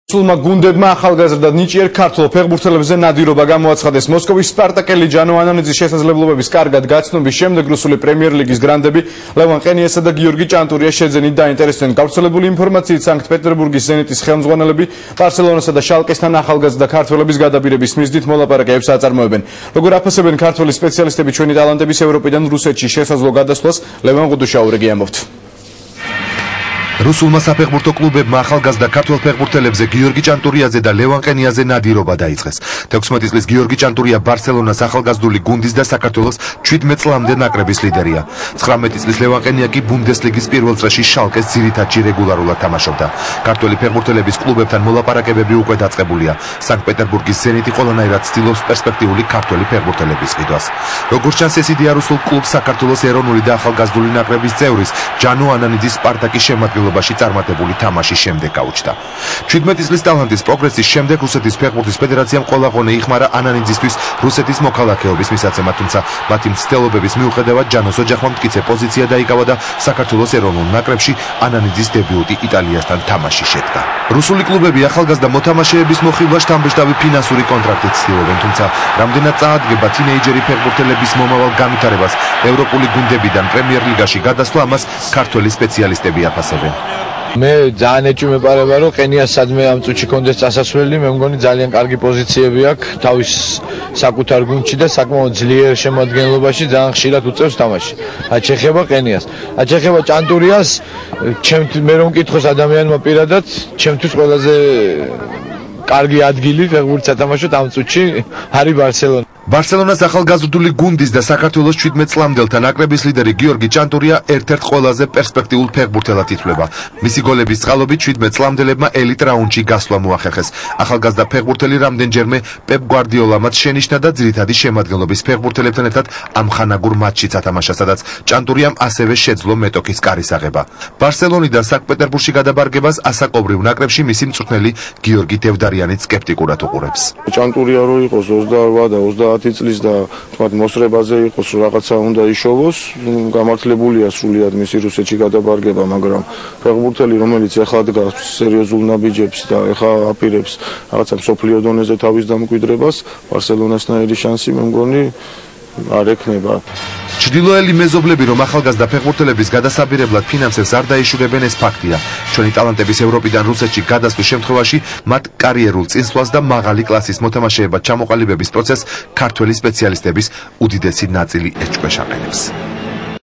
22.01.2010
რუსულმა გუნდებმა ქართველ ფეხბურთელებზე ნადირობა გამოაცხადეს